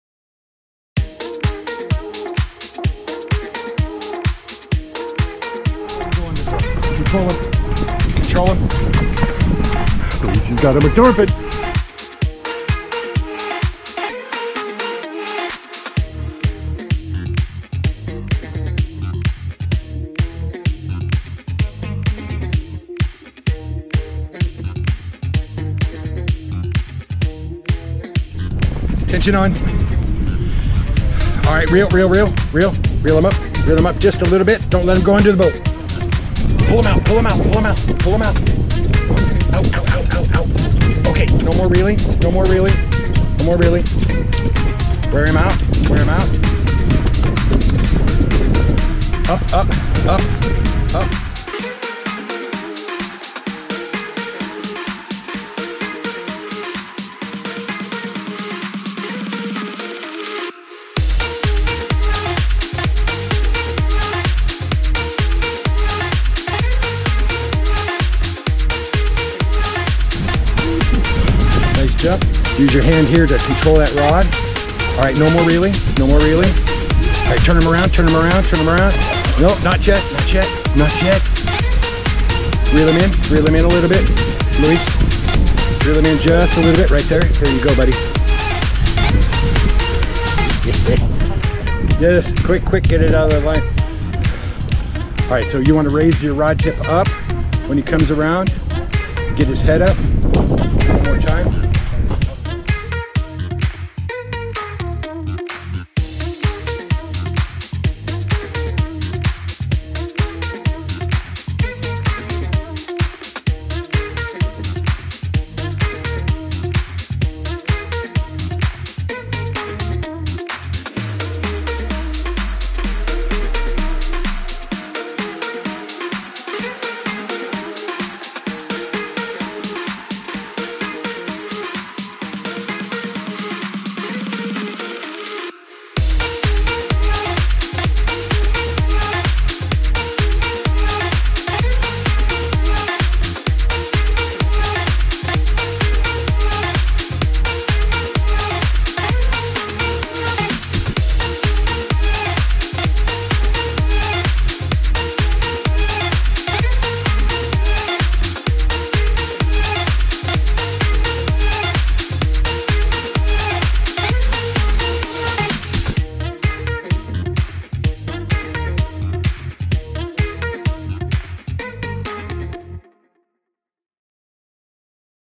Luis Battles His First Ever Tarpon - Progreso Mexico
The action and acrobatics of the Tarpon are spectacular..Luis has his hands full in this 3 minute video. Take a peak...Yucatan Mexico Inshore Fishing